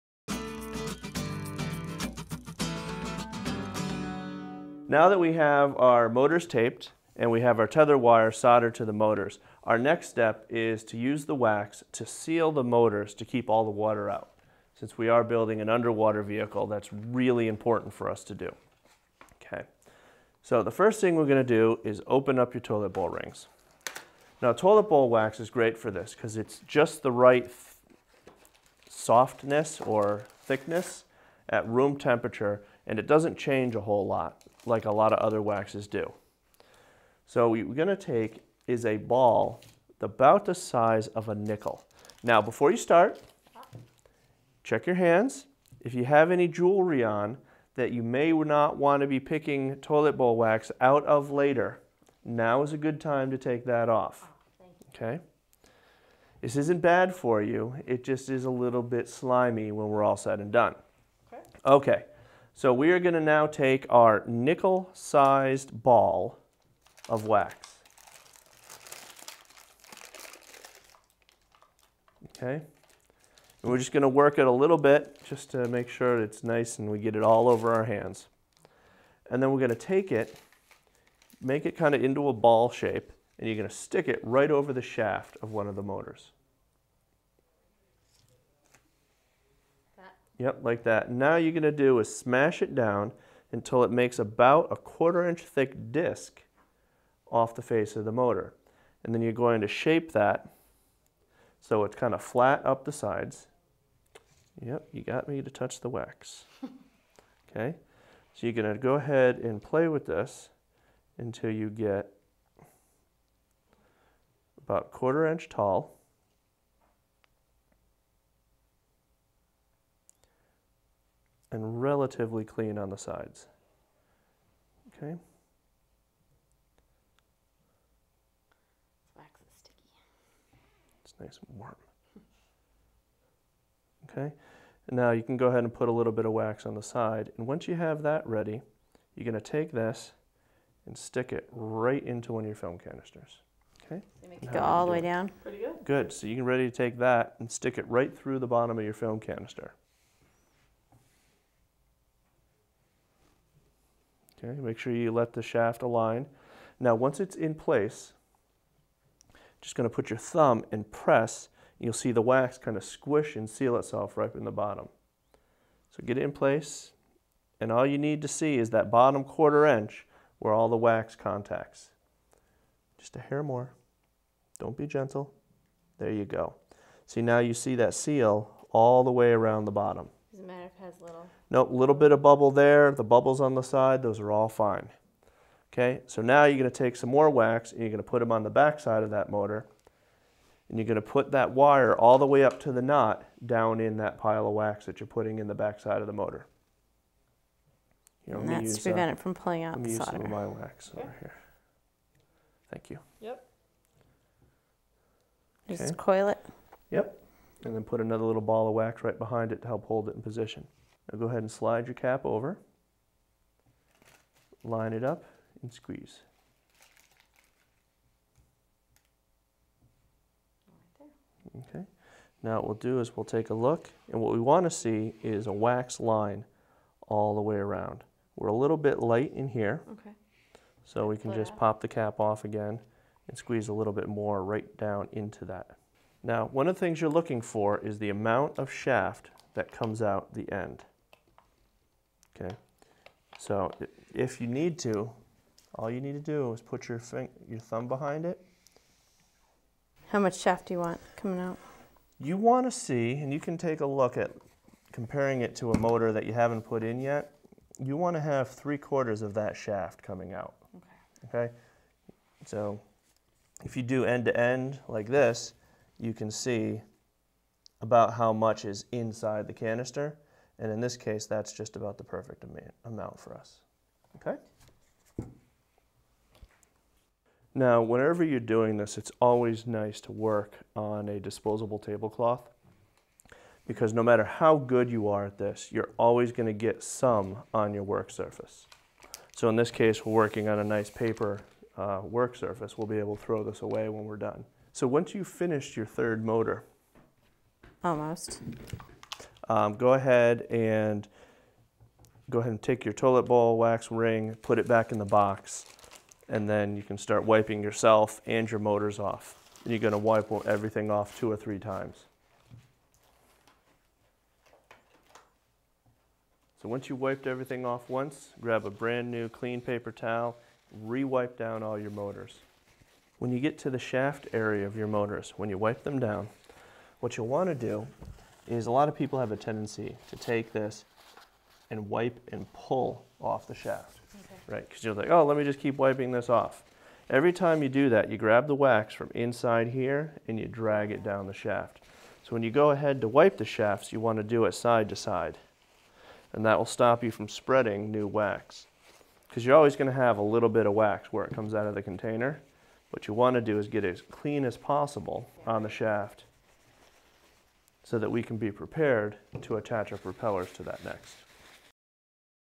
Waterproofing the Motors | SeaPerch Training Videos
These instructional videos walk you through assembling thrusters for a SeaPerch.